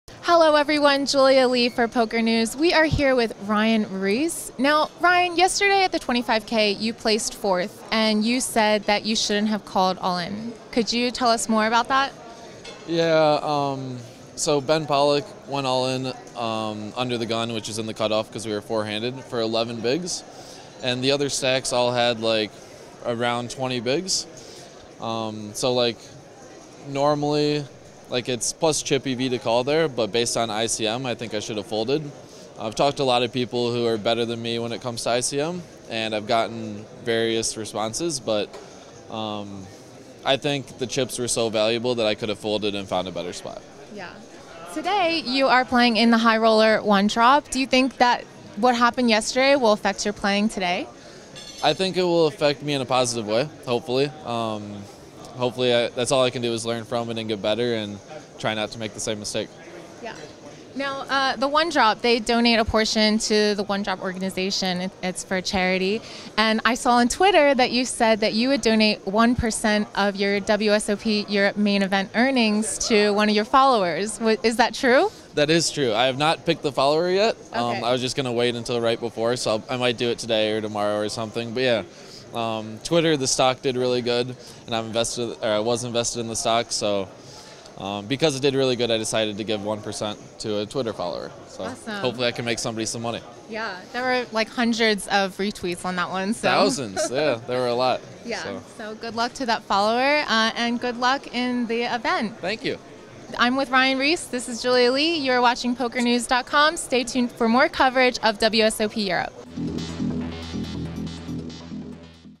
1% For Ryan Riess' Retweeter
ICM mistake? Ryan Riess finishes 4th in the €25k and talks about giving 1% to his Twitter follower for retweeting.